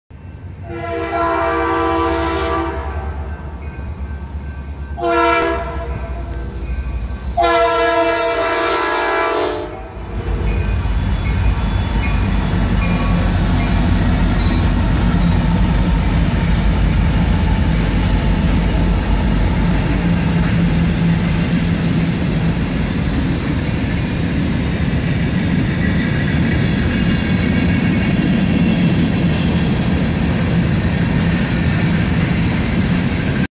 BNSF Princeton, IL
First snow of the season as a BNSF freight races past the Amtrak Station and Coffee Cup diner in Princeton, IL